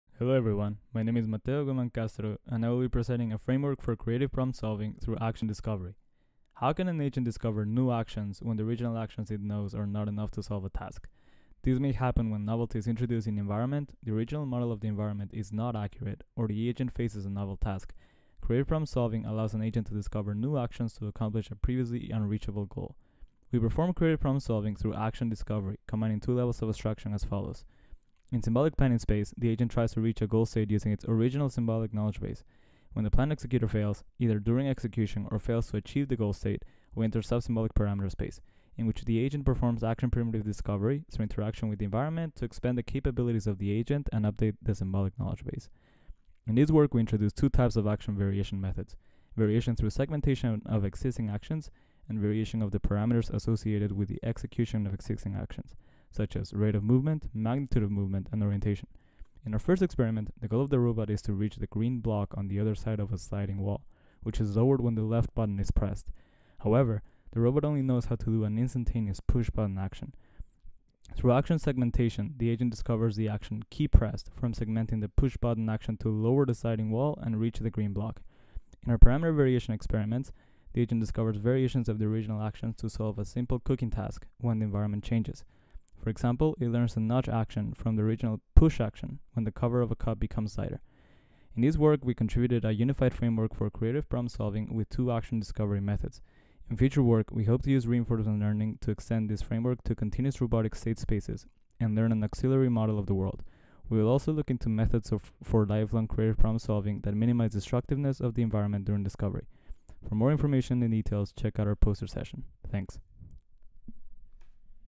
Talk: A Framework for Creative Problem Solving Through Action Discovery. RSS 2021 DNR ROB Workshop
Pre-recorded talk for A Framework for Creative Problem Solving Through Action Discovery, presented at the RSS 2021 Workshop on
Declarative and Neurosymbolic Representations in Robot Learning and Control.